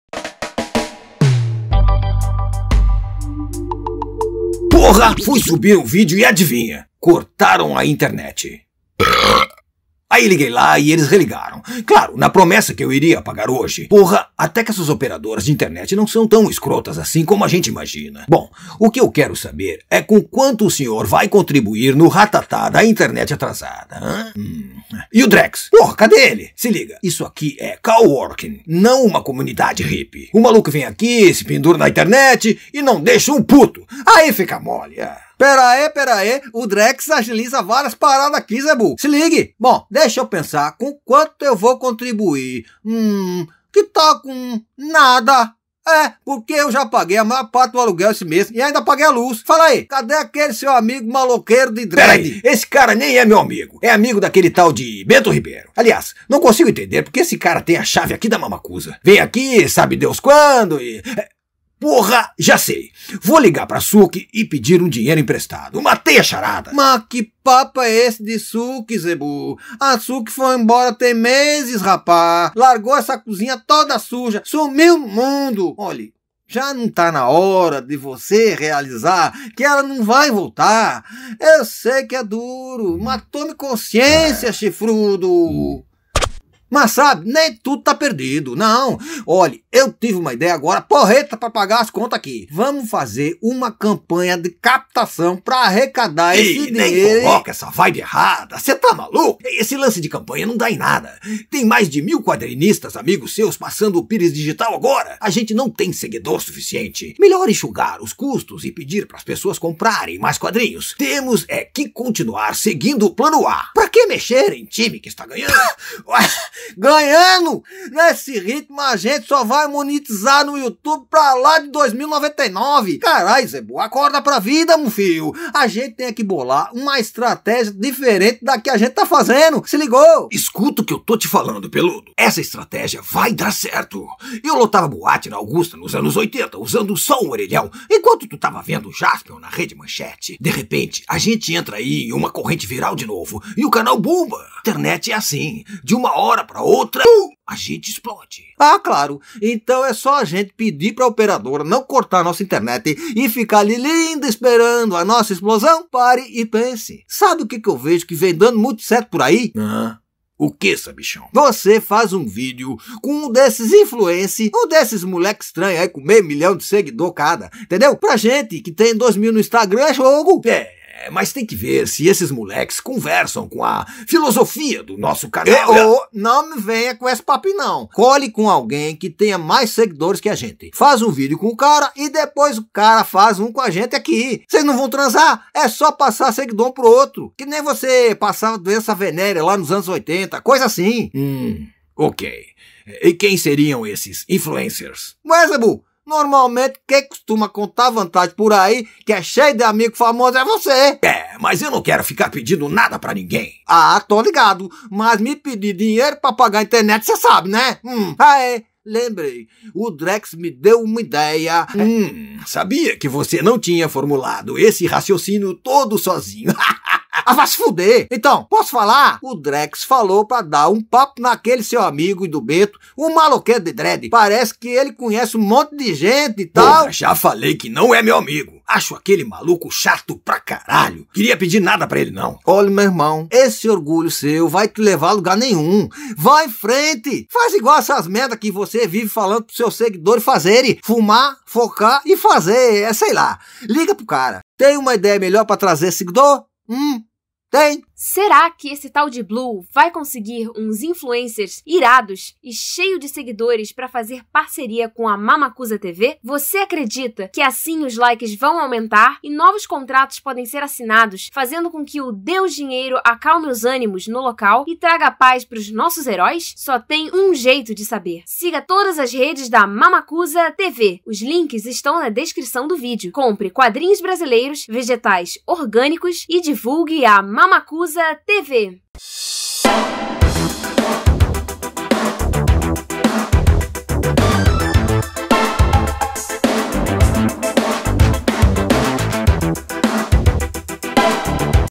BEAST DORES MAMAKOOSA TV EPISÓDIO 02 (S01E02)
É... parece que a crise bateu na porta da Mamakoosa TV. Você pode não acreditar mas é preciso passar por uma crise as vezes para repensar suas estratégias. Mais um episódio da série Beast Dores da Mamakoosa TV

Página da Banca do Cícero no facebook

Perfil do Zebu

Intagram